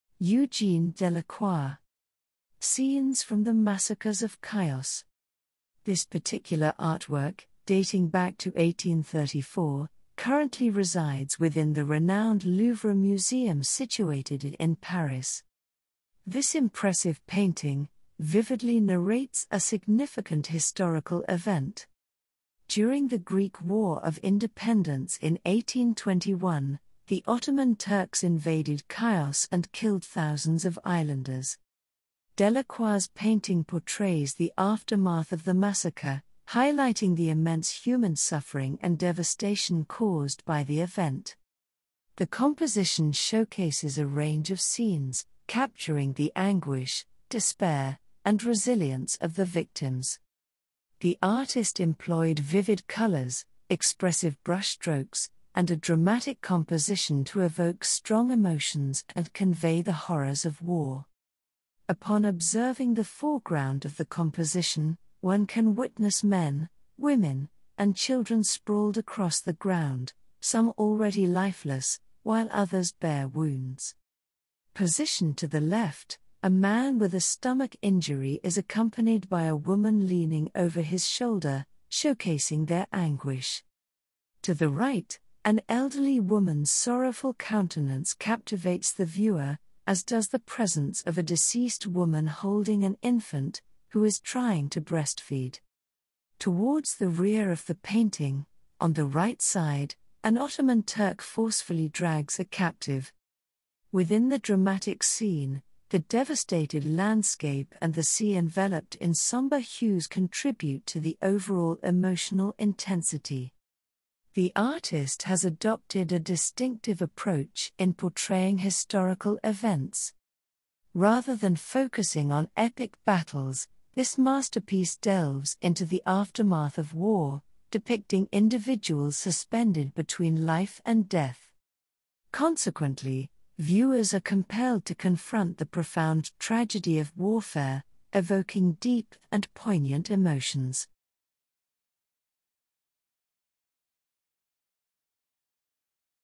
Scenes from the Massacres of Chios, Eugene Delacroix
This video, showcases a painting from 1834, housed in the Louvre Museum, Paris, depicting the aftermath of the Ottoman invasion of Chios during the Greek War of Independence in 1821. Delacroix's work vividly captures the human suffering and devastation caused by the massacre, portraying scenes of anguish, despair, and resilience. The composition focuses on individuals, including men, women, and children, amidst a devastated landscape and somber seascape, inviting viewers to confront the profound tragedy of warfare.